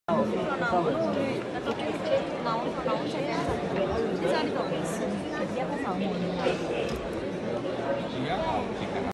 Grateful for the love you showed us during world book fair 2020.
Hi Guys! Key2practice was at The World Book Fair 2020. Lakhs of people visited the fair. It was a humbling experience to present our work in front of you all. Thank you so much for appreciating our hard work.